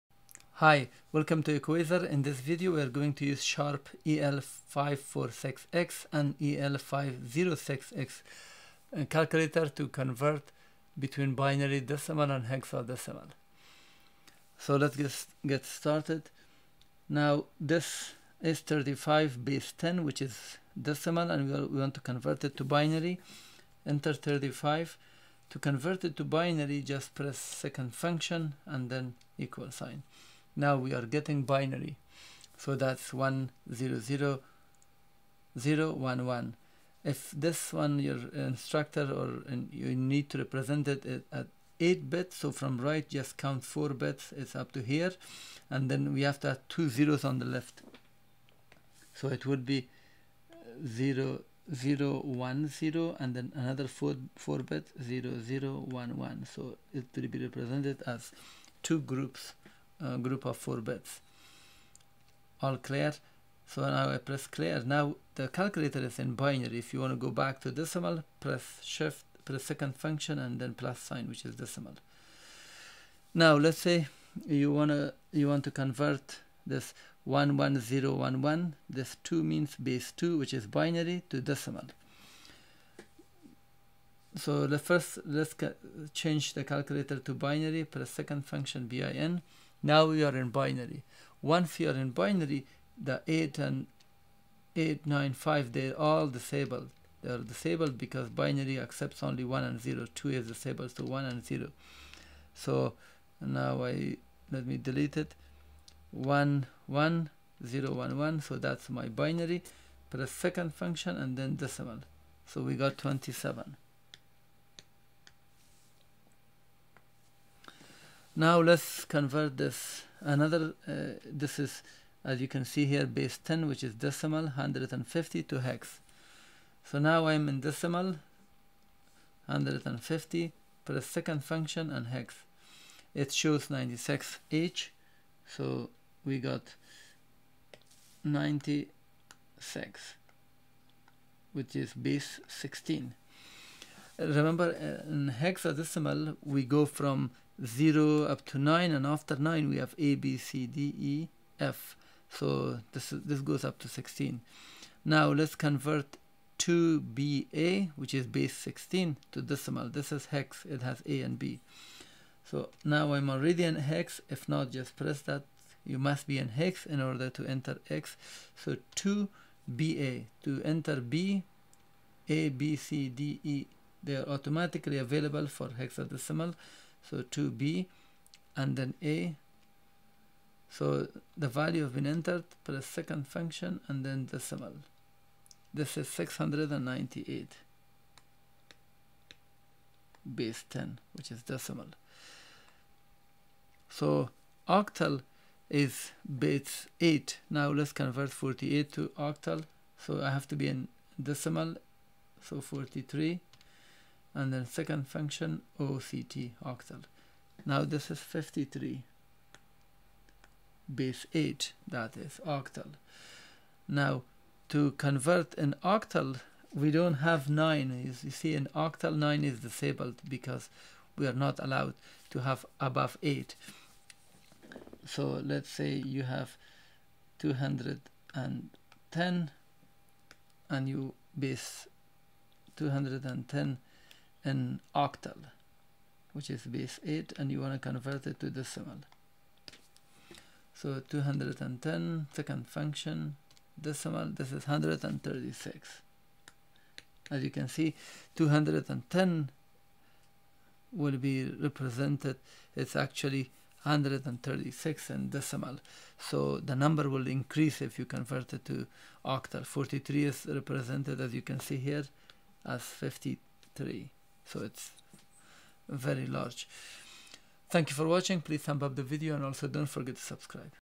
Sharp EL-546X EL506X convert Binary, Decimal, Hexadecimal and Octal
This video will show you how convert Binary, Decimal, Hexadecimal and Octal to one another.